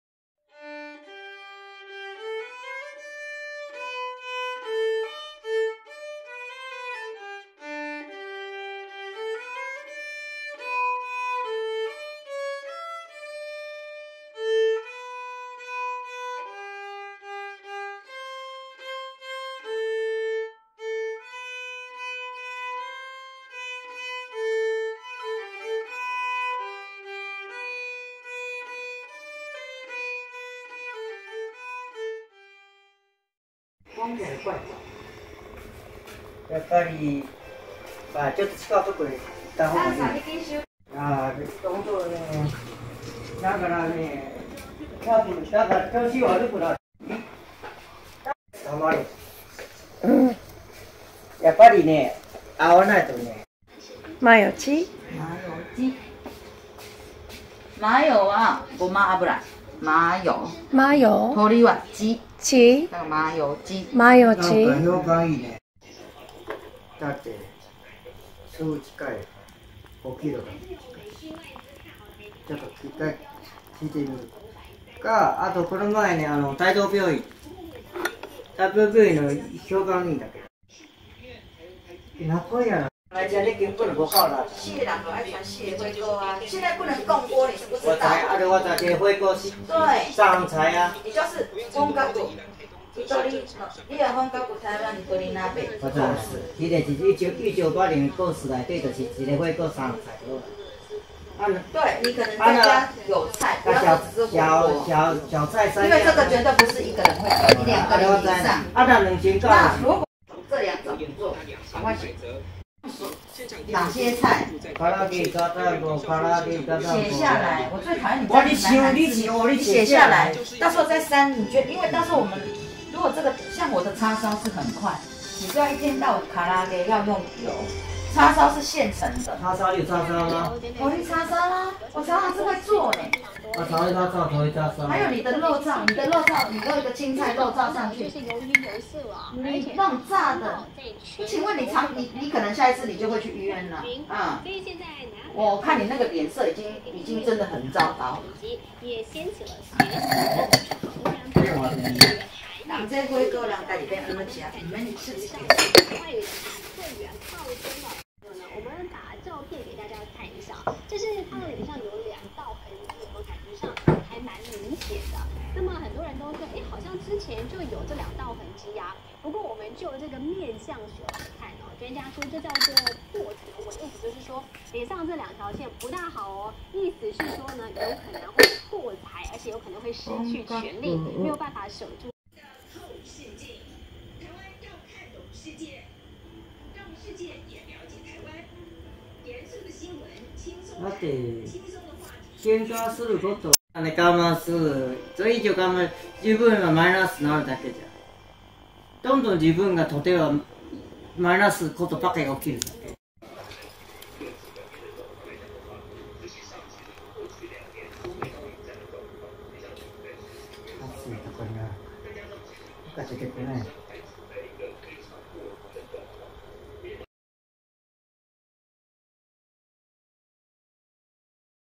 【麻油鶏鍋】 4K QQ台湾美食 ひとり鍋定食 麺線 台湾ソーセージ with Sony XPERIA1 Ⅱ#愛知ごはん
QQ台湾美食 名古屋市
台湾の屋台料理が楽しめます
どれも非常に美味
台湾がご出身の店主の方が気さくで優しい

撮影使用端末 Sony Sony XPERIA1 Ⅱ Cinema Pro